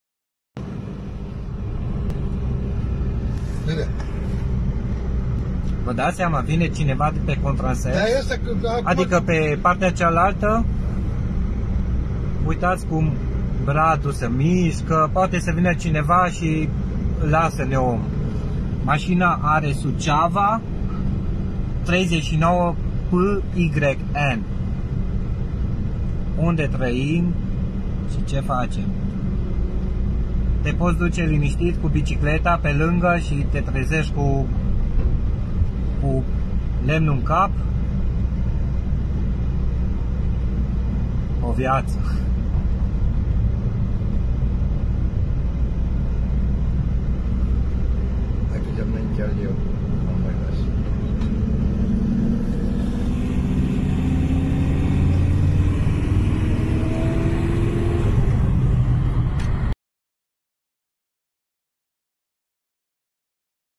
Transport periculos de bușteni
Imagini surprinse de un turist aflat în trecere prin județul Suceava, zona comunei Crucea